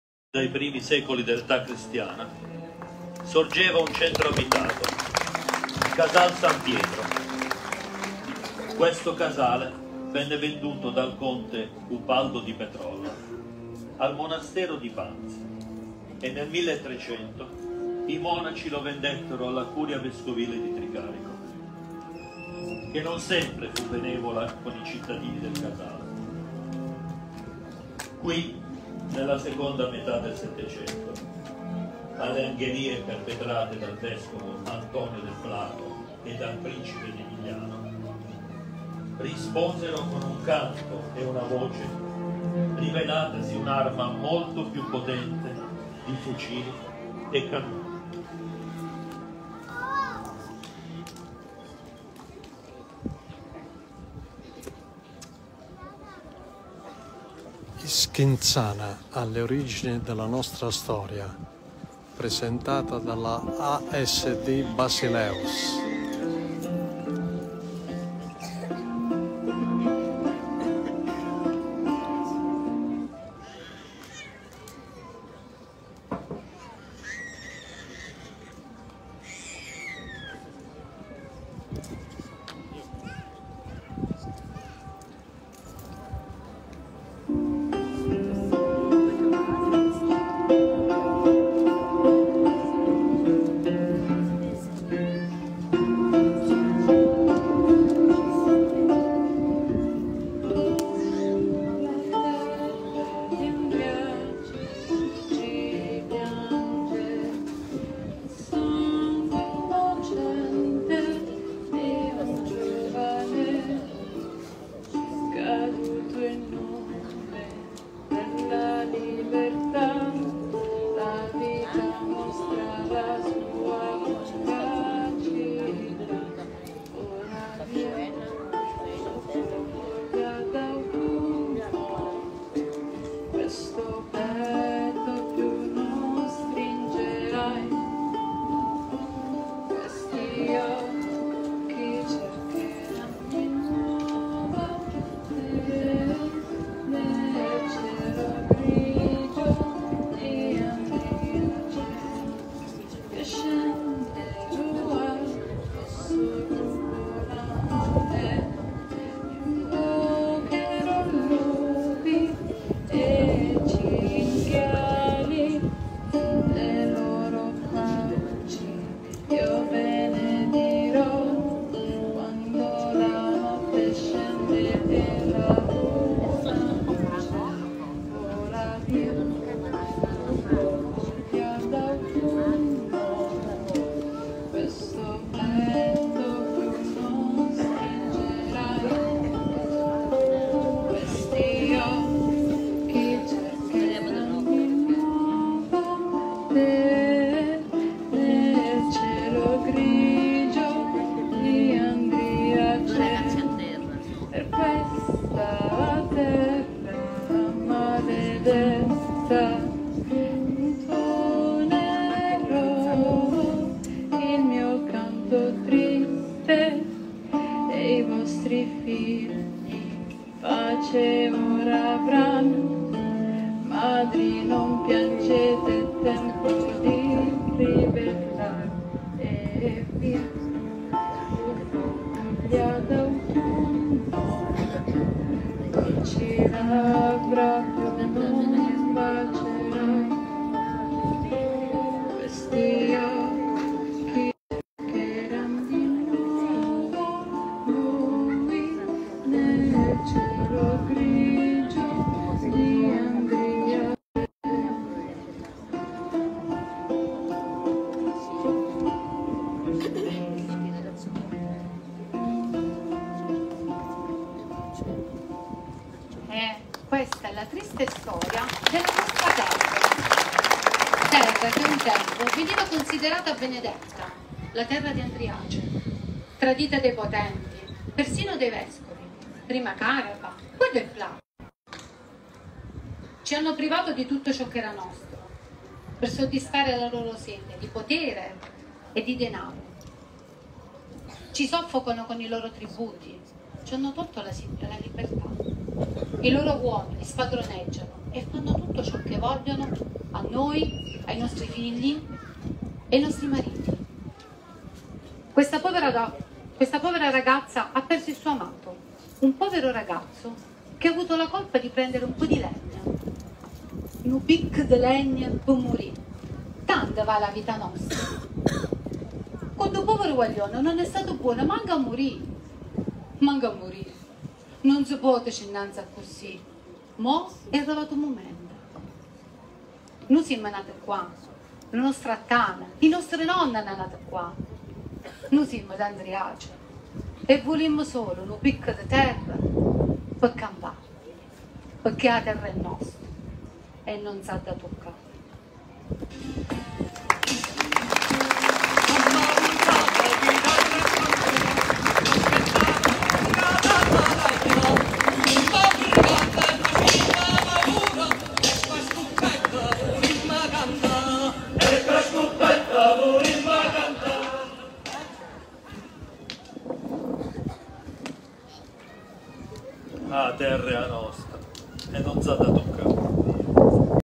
"HISKINZANA, ALLE ORIGINI DELLA NOSTRA STORIA". LA RAPPRESENTAZIONE DELL'ASD BASILEUS
LE RADICI DI SCANZANO JONICO.
ATTORI LOCALI NON  PROFESSIONISTI ED IL PUBBLICO DELLE GRANDI OCCASIONI PER IL LAVORO DI PASQUALE RIMOLI, MARTINO IANNIBELLI E PAOLO GRECO